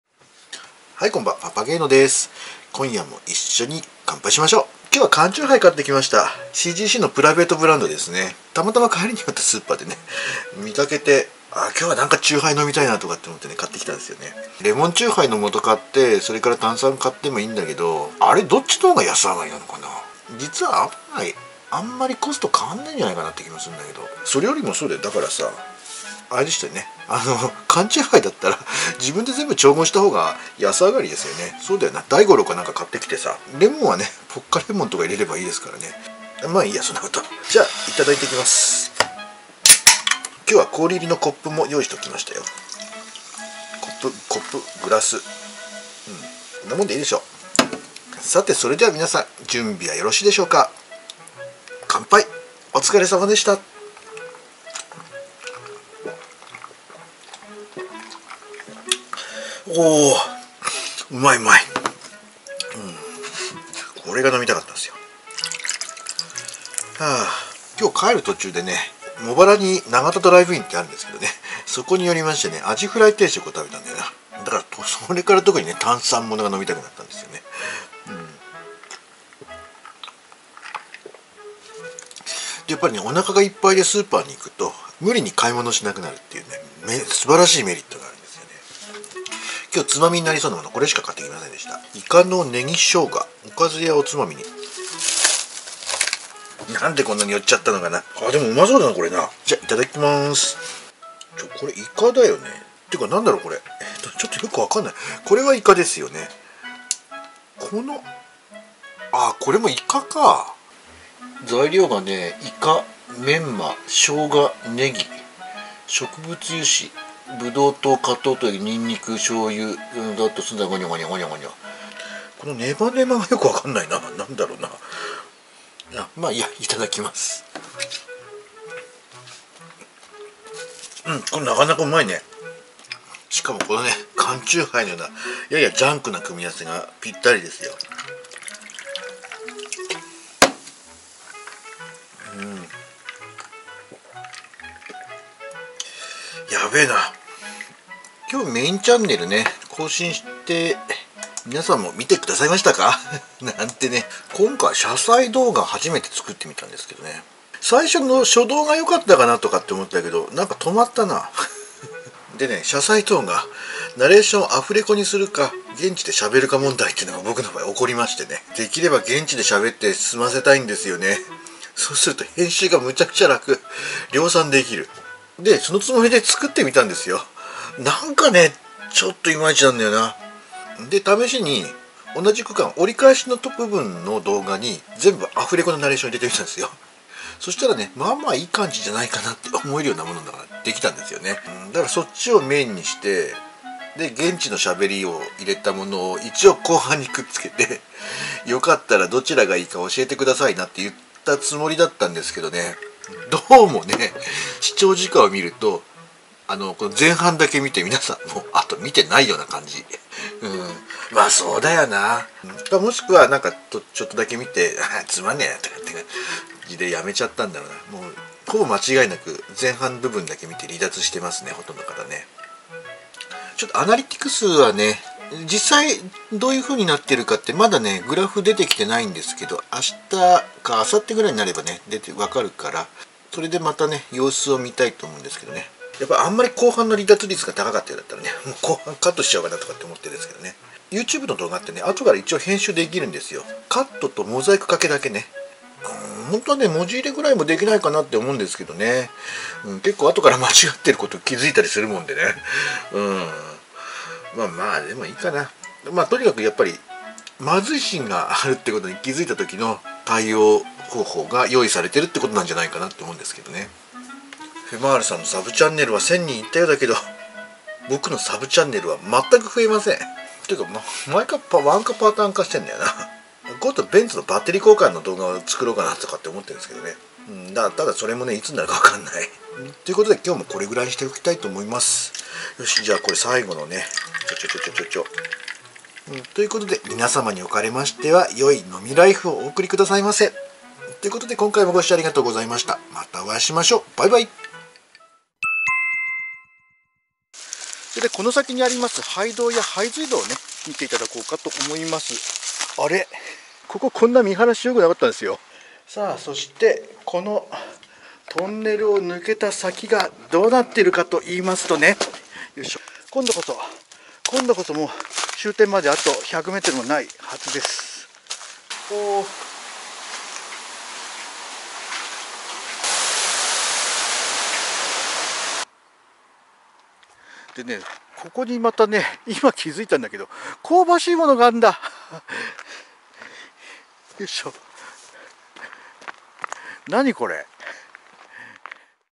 【おっさんと乾杯#14】車載動画も楽じゃないっすね、君津で探索してきた時の様子もチラ見せします
※メインチャンネルもどうか宜しくお願いします！！
【道にあるちょっと古いもの/穴リストチャンネル】